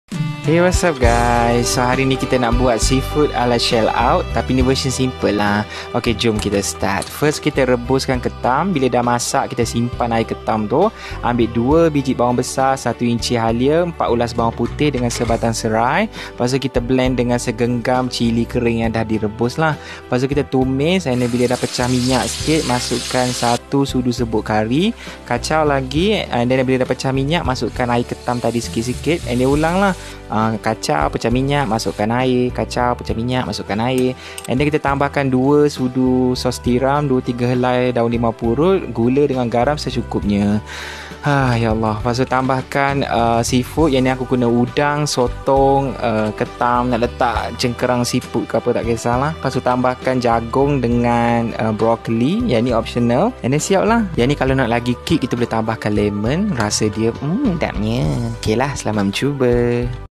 Puasa Day 15 : Seafood ala ala Shell Out
Bahan2/Ingredients:
[PART 1]
🌕2 ekor ketam, direbus, simpan air rebusan (2 crabs, boiled, keep the broth)
[PART 2]
BLEND ALL
🌕2 biji bawang besar (2 onions)
🌕1 inci halia (1 inch ginger)
🌕4 ulas bwg putih (4 cloves garlic)
🌕1 btg serai (1 lemon grass)
🌕1 genggam cili kering, direbus (1 handful dried chilli, boiled)
[PART 3]
🌕1 sudu rempah kari daging (1 tbsp curry powder)
🌕2 sudu sos tiram (2 tbsp oyster sauce)
🌕2-3 helai daun limau purut (2-3 kaffir lime leaves)
🌕Gula (sugar)
🌕Garam (salt)
🌕Seafood kegemaran anda (u
your fav seafood)
🌕Jagung,direbus (corn, boiled)
🌕Broccoli, mentah (raw)
🌕Lemon